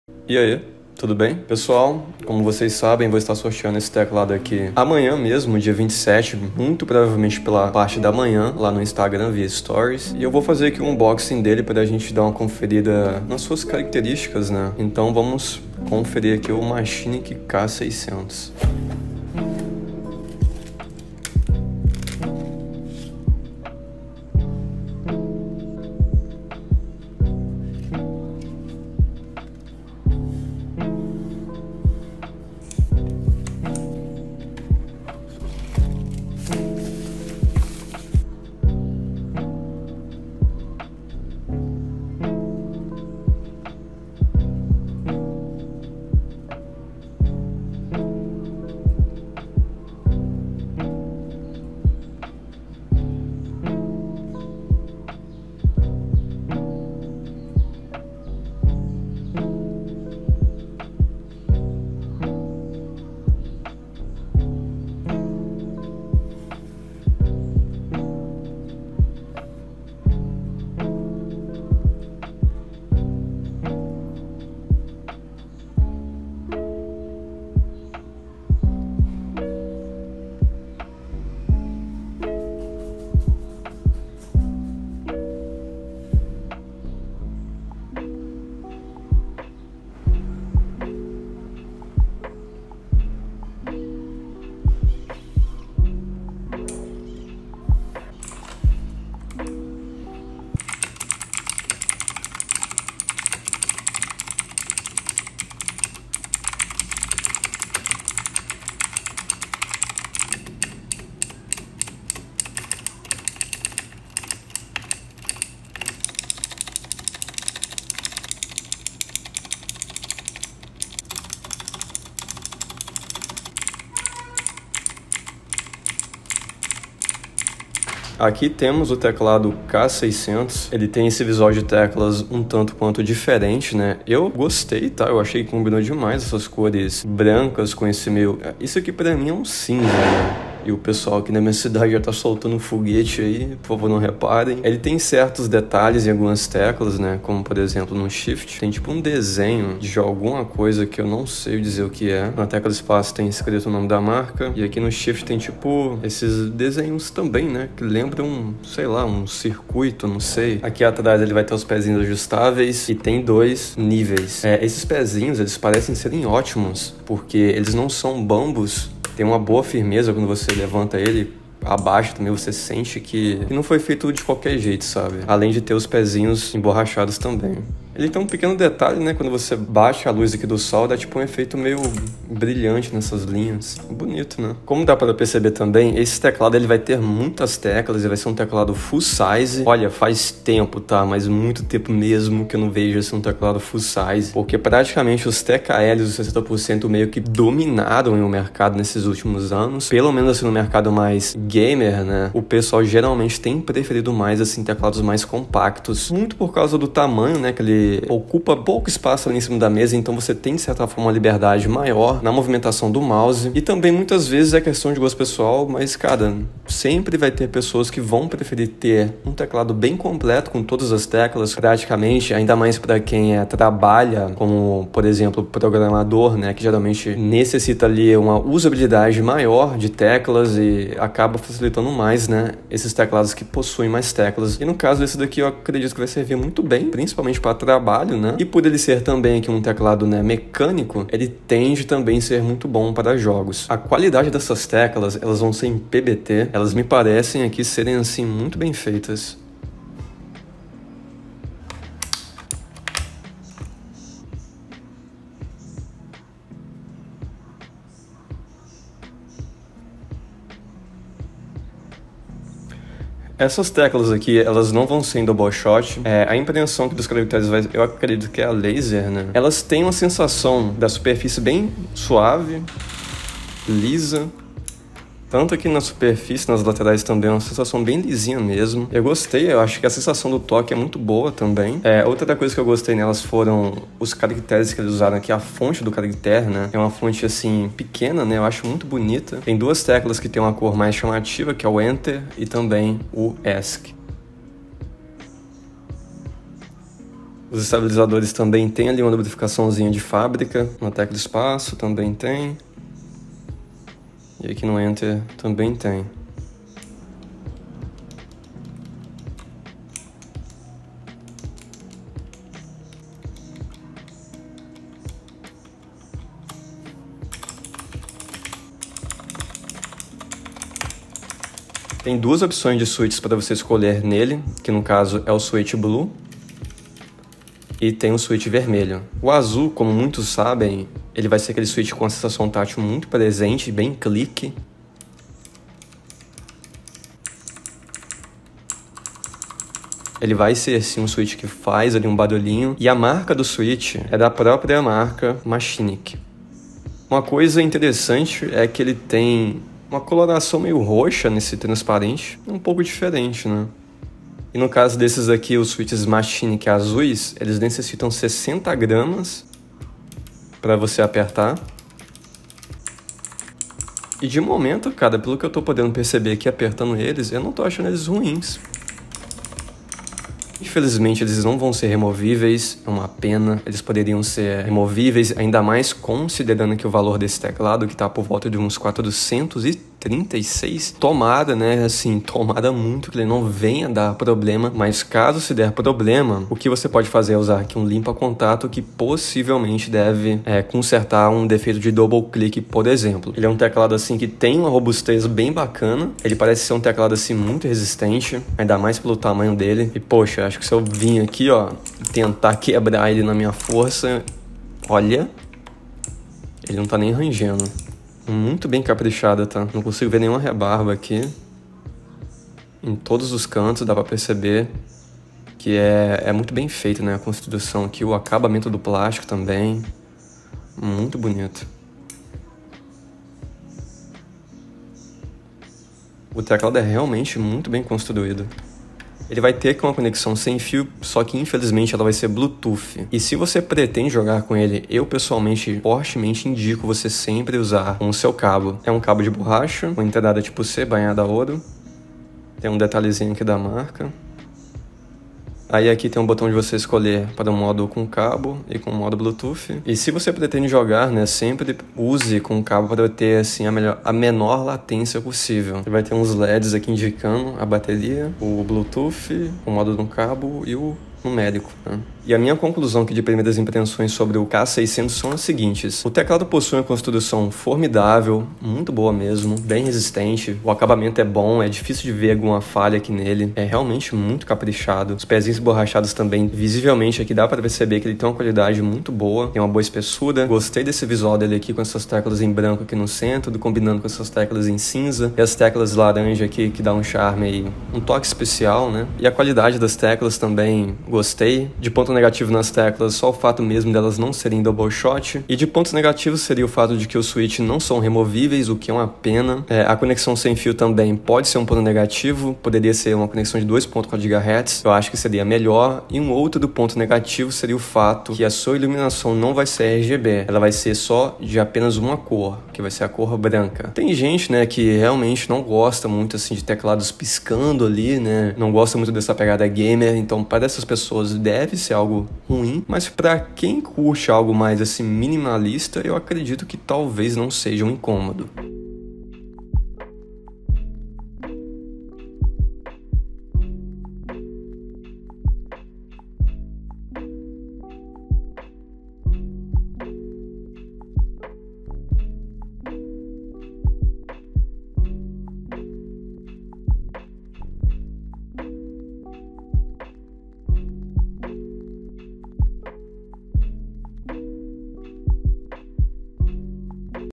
Unboxing Machenike k600 Switch Azul - Teclado do sorteio
video sobre o teclado mecanico machenik k600 que sera sorteado amanha dia 27 pela parte da manhã
Link machenike k600
NOVO cupom no Aliexpress para novos usuários '' fraguasn3 '' 3$ dólares off para compras acima de 10$ dólares